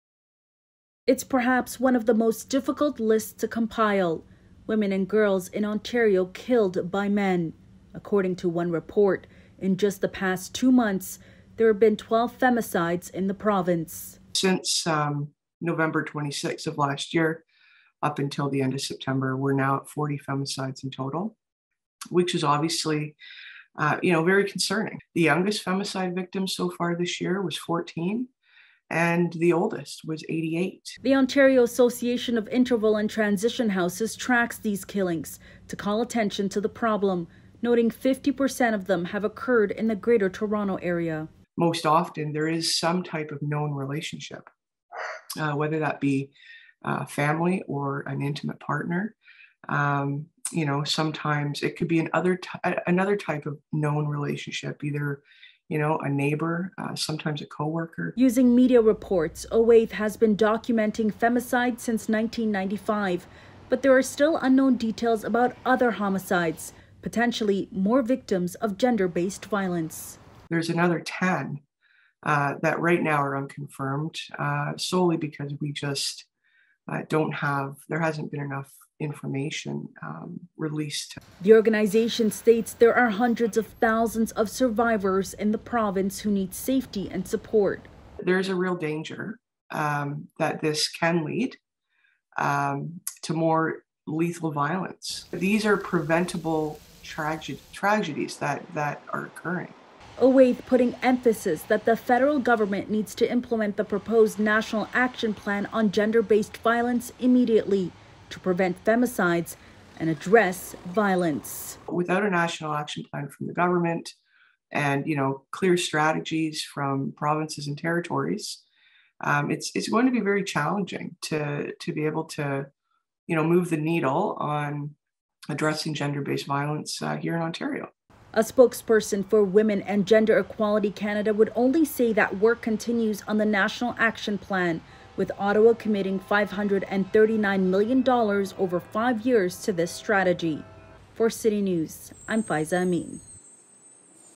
Calls for a national strategy to end gender-based violence
The Ontario Association of Interval and Transition Houses is calling on the government to release the proposed national strategy to address gender-based violence.  Faiza Amin speaks with OAITH about the concerning list of femicides in Ontario.